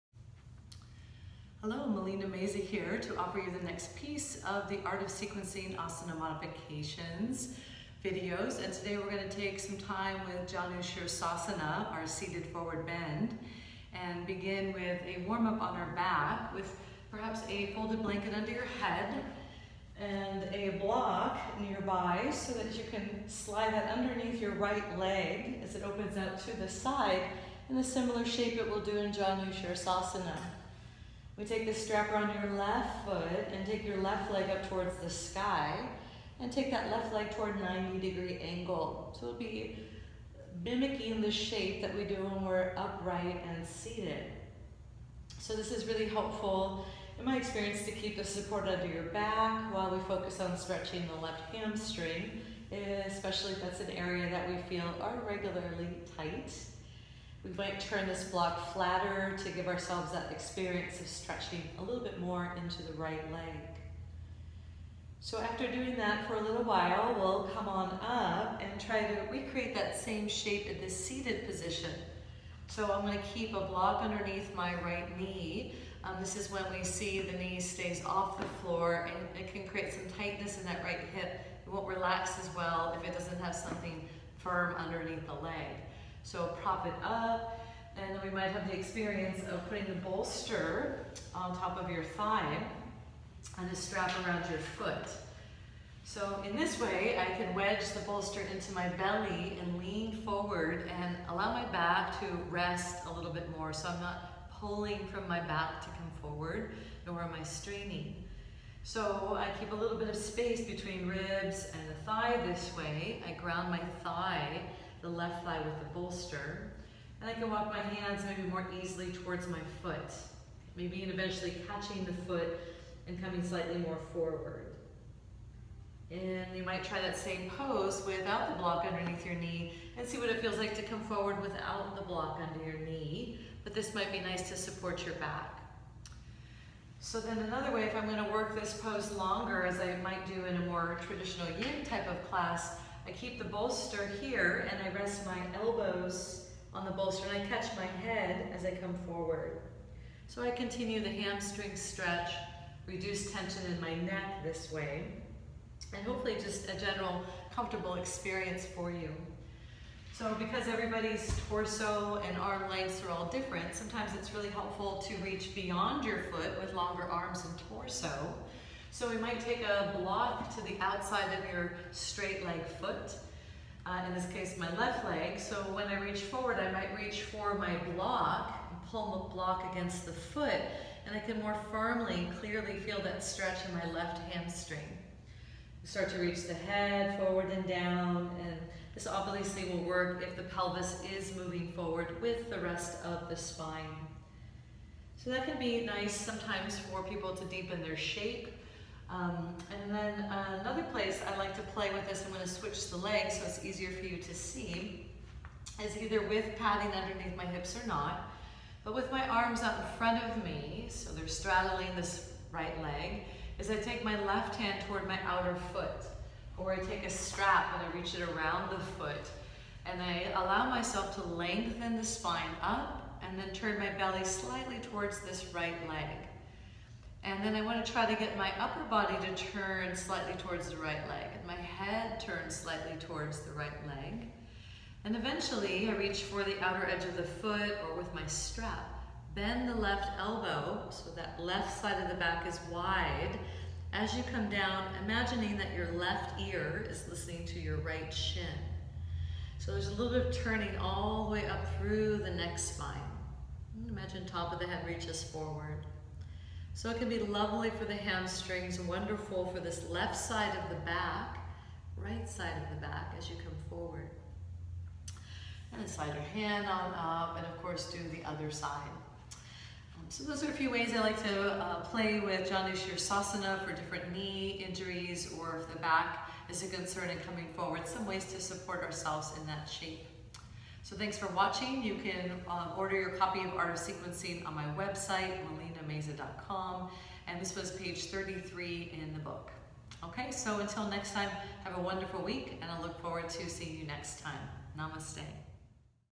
Janu Sirsasana
Janu Sirsasana variations from Art of Sequencing – Volume 3 – Asana Modifications Book.